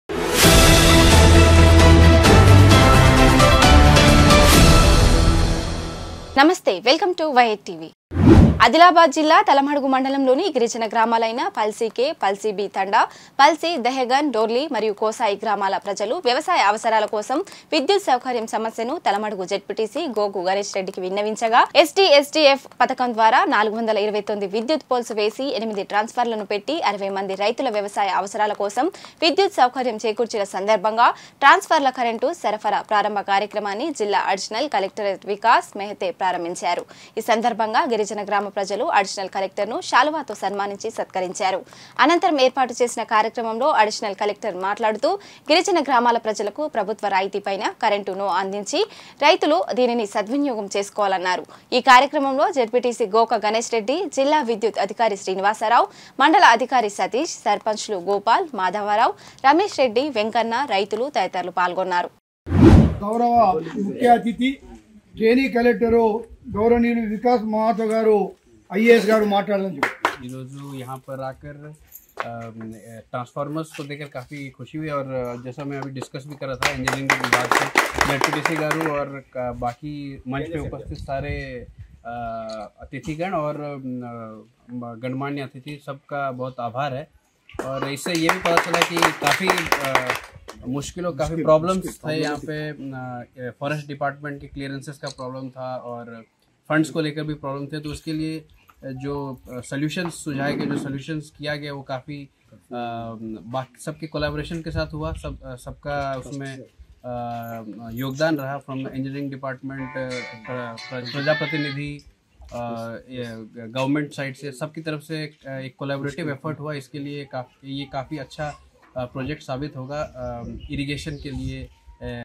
60 మంది రైతులకు ఊరట..!
60 మంది రైతులకు ఊరట..! ||  @y8tvnews
జాతీయ తెలుగుదేశం పార్టీ అధ్యక్షులు శ్రీ నారా చంద్రబాబు నాయుడి అక్రమ అరెస్టును ఈ నిరసిస్తూ నియోజకవర్గ కండ్ర కులస్తుల ఆధ్వర్యంలో చేపట్టిన రిలే నిరహార దీక్ష కార్యక్రమం 25వ రోజుకి చేరింది.. ఈ కార్యక్రమంలో..శ్రీకాకుళం నియోజకవర్గం తెలుగుదేశం పార్టీ ఇంచార్జి మాజీ MLA గుండ లక్ష్మీదేవి ఈ దీక్షకు పద్దతు తెలపడం జరిగింది... ఈ సందర్భంగా ఆమె మాట్లాడుతూ. వైసీపీ ప్రభుత్వం చంద్రబాబుని అన్నివిధాలుగా అవమాన పరచాలని..ఎన్ని అవమానాలు పరిచిన..మా కార్యకర్తలు ఉన్నత వరకు మా నాయకుడికి ఏమీ కాదని..ఆమె ఎచ్చరించారు..ఈ కార్యక్రమంలో నియోజకవర్గం టిడిపి నాయకులు,కార్యకర్తలు పాల్గొన్నారు. _Y8 TV NEWS is a dedicated News Channel with a vision that Enhancing Accountability and Responsibility of the System by empowering people through credible News.